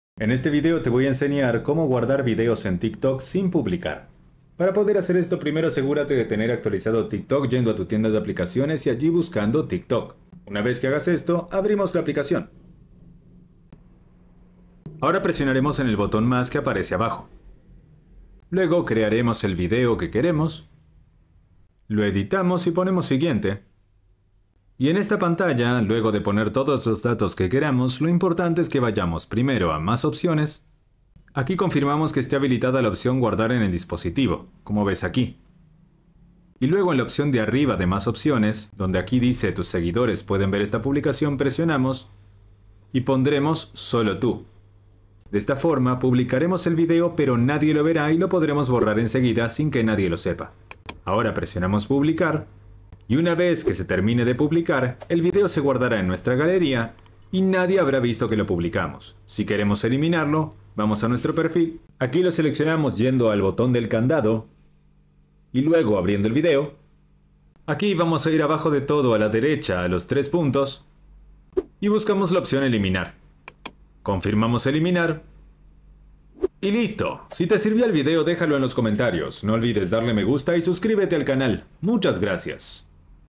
Como guardar videos en TikTok sin publicar (GUÍA COMPLETA)
Aprenderás como guardar videos en tiktok sin publicar. Sirve para como grabar videos en tiktok sin publicar.